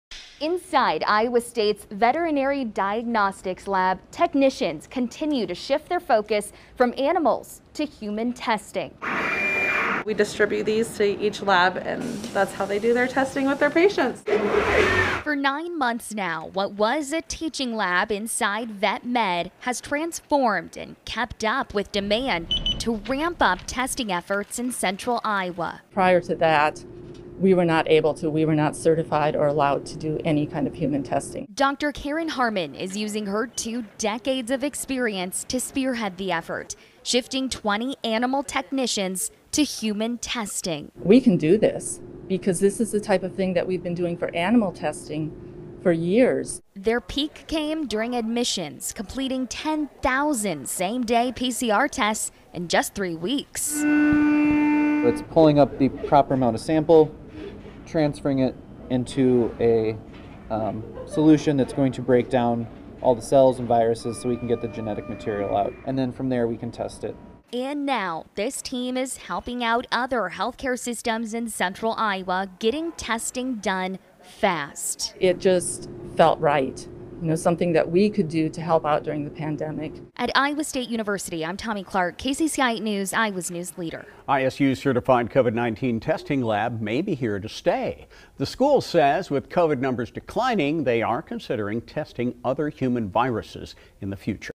Tour Iowa State University’s COVID-19 testing lab
When the COVID-19 pandemic reached Iowa in 2020, the Iowa State University Veterinary Diagnostics Lab had to shift from animal outbreaks to humans.